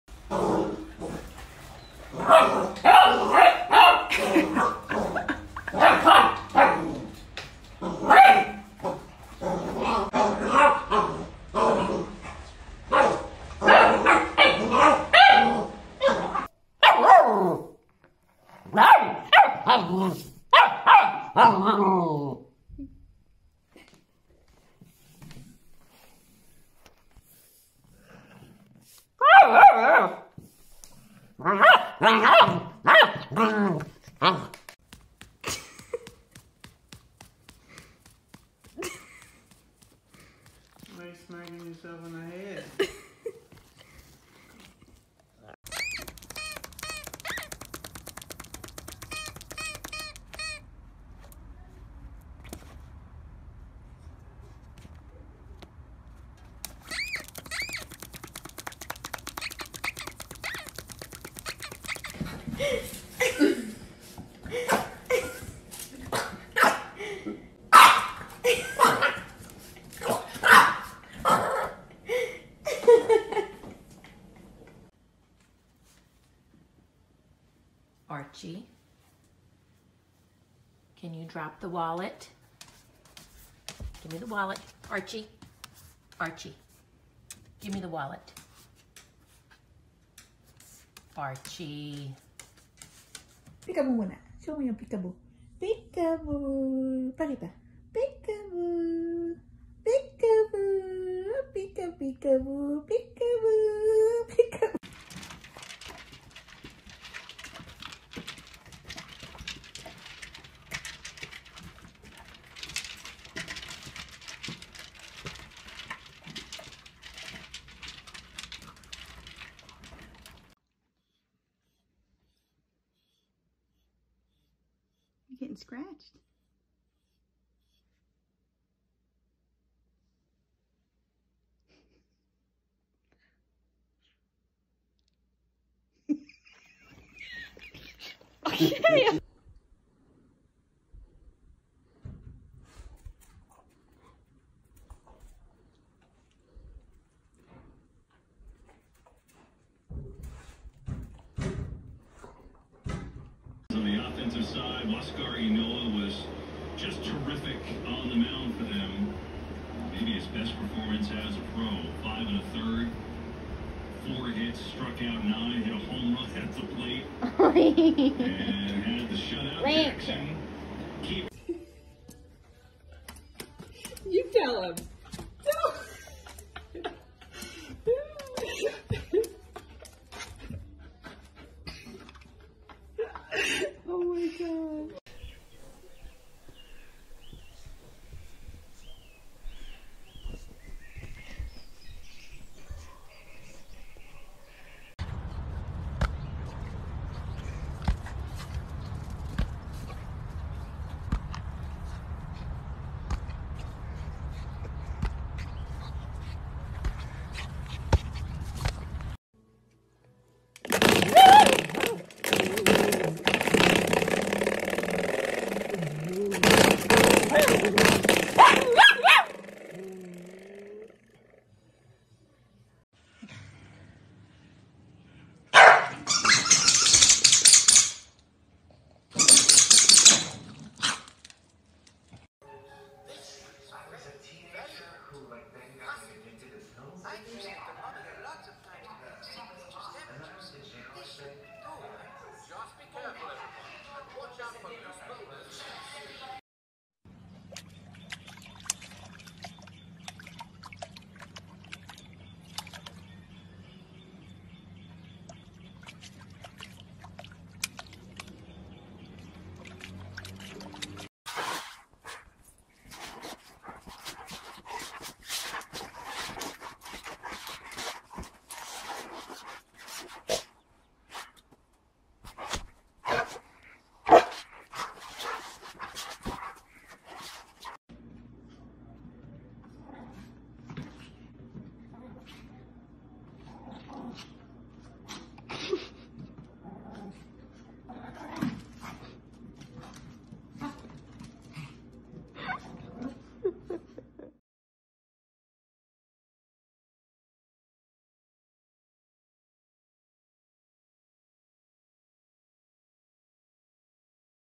Funny Dogs And Cute Puppies - Cutest Dog Videos 2021
These funny dogs and cute puppies will definitely make your day more positive! Cutest dog videos include Shiba Inu puppy playing with pillow, funny
Dachshund loves her smart device, Beagles having fun with broom and more.